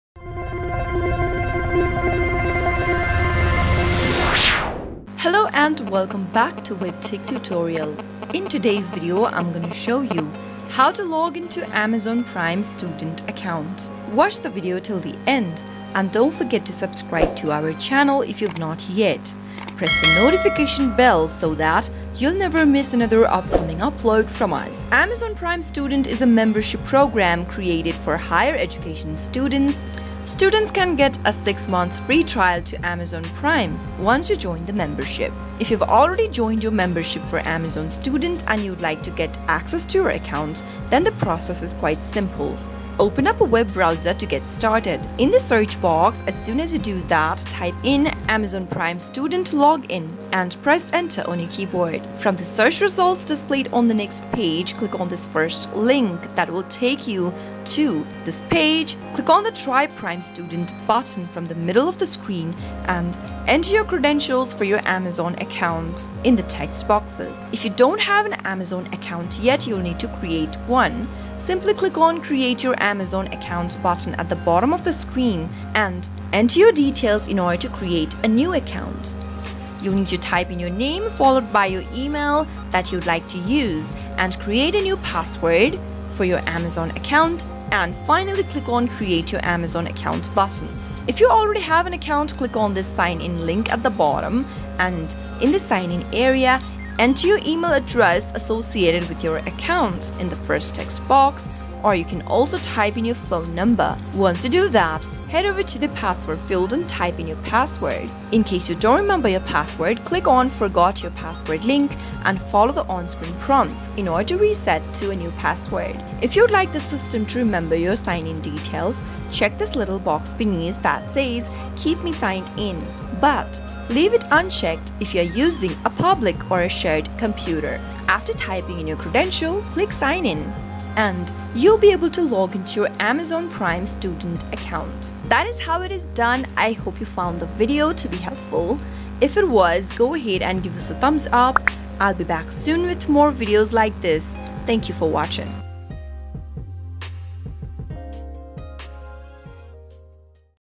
Amazon Prime Student Login: How To Login into Amazon Prime Student Account (2022)
How To Login into Amazon Prime Student Account (2022)

Amazon Prime Student Sign Up (6 Months Free)

Confused about how to login into Amazon Prime Student account? In this video, we'll learn the exact steps on logging into Amazon Prime Student account. If you've already signed up for amazon student, log into your amazon prime account. If you don't have an amazon account yet, you'll need to create one. In order to create a new account, you need to type in your name followed by the email that you'd like to use, create a password for your amazon account and finally click on the "create your amazon account" button at the bottom of the screen.

CHAPTERS
0:00 - Intro
0:24 - Steps To Login into Amazon Prime Student Account
2:16 - Outro